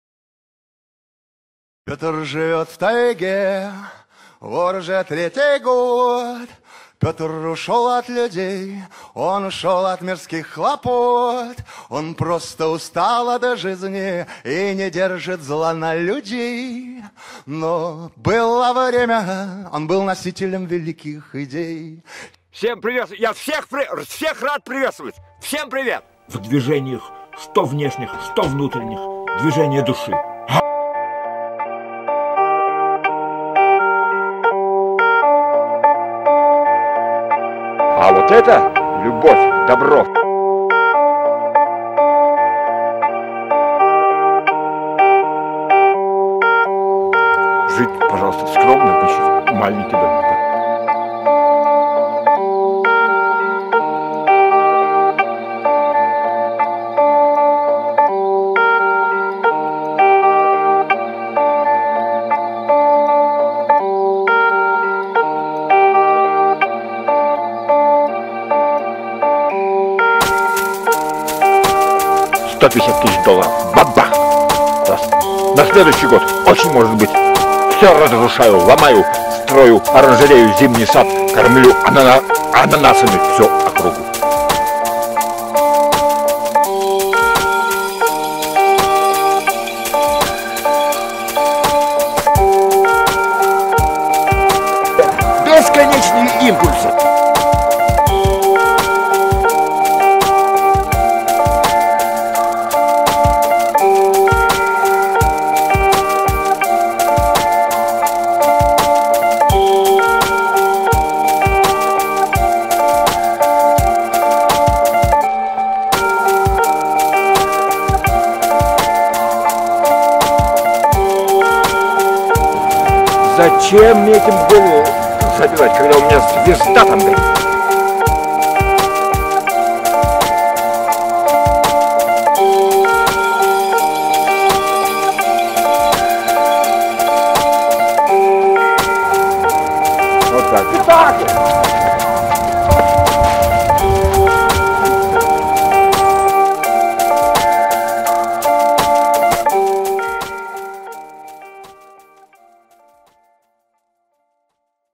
Убежище | Мария Майорова | Портфолио студентов | Школа дизайна НИУ ВШЭ
Серия иллюстраций, посвященная теме "Убежище". Изображения выполнены в различных техниках и разбиты на категории (портрет, пейзаж, натюрморт и животное). В качестве предмета – трава, как возможный материал для создания убежищ, пейзаж – уединенные домики и шалаши, животное – броненосец, а портрет – Петр Мамонов.